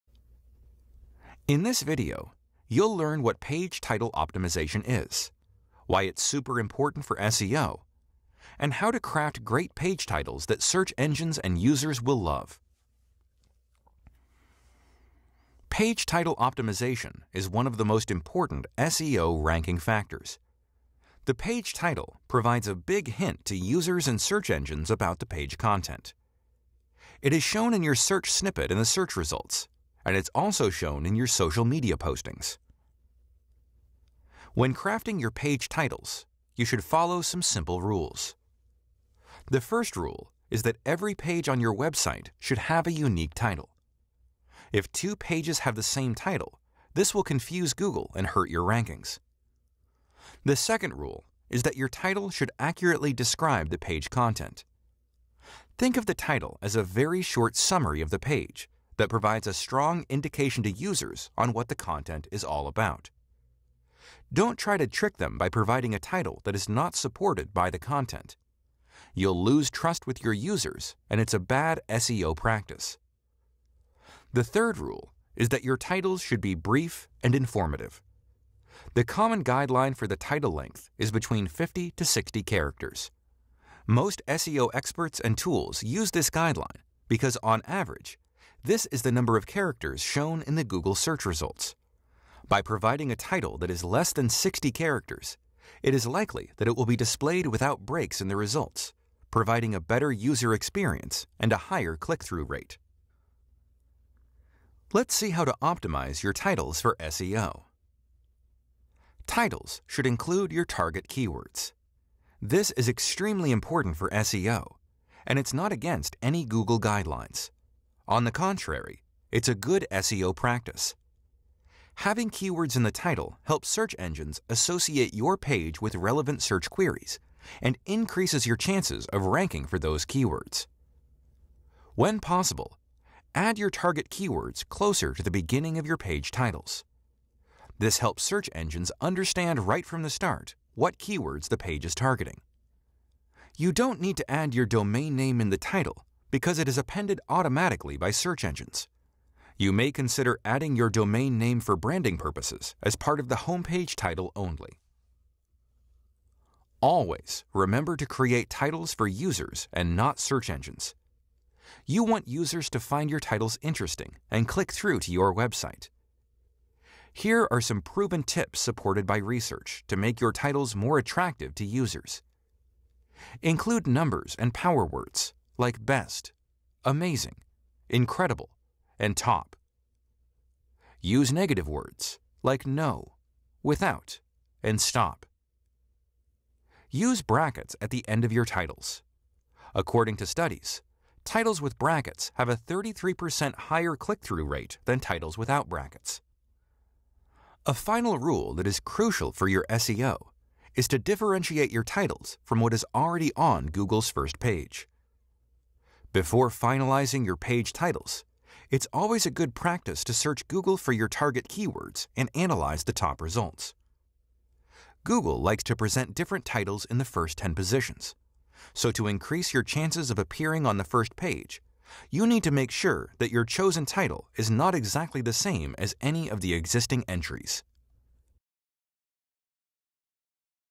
SEO Page Title - How to optimize Title Tags for SEO
👍 In this video, you’ll learn actionable techniques to fully optimize your page titles for SEO. Optimized titles can lead to better SEO rankings and generate more traffic to your website.

By the end of this video, you'll know:

➜ What page title optimization is
➜ Why page titles are super important for SEO
➜ How to craft great page titles that search engines and users will love.

⏰TIMESTAMPS⏰
00:00 - Intro
00:35 - Page Title Optimization
01:05 - Page Title Length
01:46 - Keyword Usage in Page Titles
02:37 - Crafting the perfect page titles for users